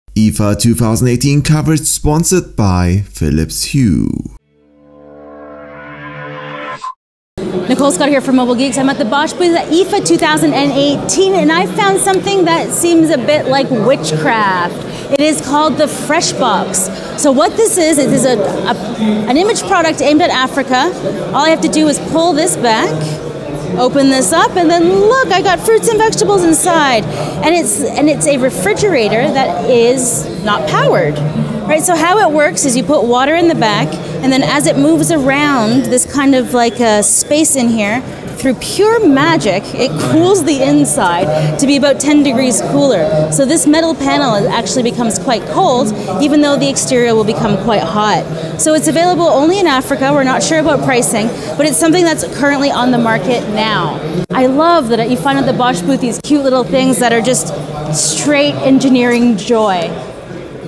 Food in Africa - Freshbox from Bosch keeps groceries fresh with inexpensive tech.
A social project from Bosch helps people in Africa to store food  without using a lot of expensive tech, since everything you need for the freshbox ist just a little bit of Water and the surrounding temperature and you get a 10 degrees cooler inside. It´s an inexpensive fridge without much tech.